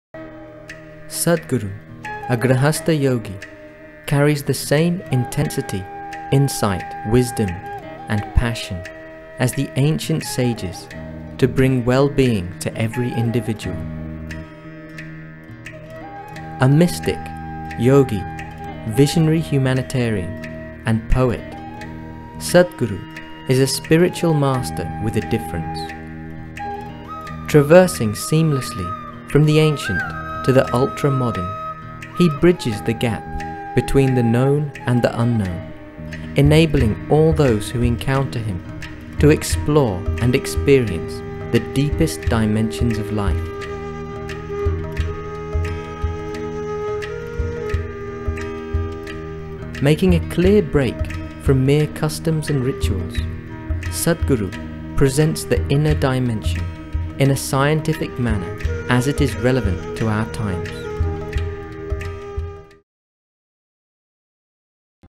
Sadhguru introduction | Sadhguru wisdom
Yogi mystic and visionary sadhguru is a spiritual master with a difference . An arresting blend of profundity and pragmatism his life and work serves as a reminder that yoga is a contemporry  science , vitally relevant to our times

Sadhguru Wisdom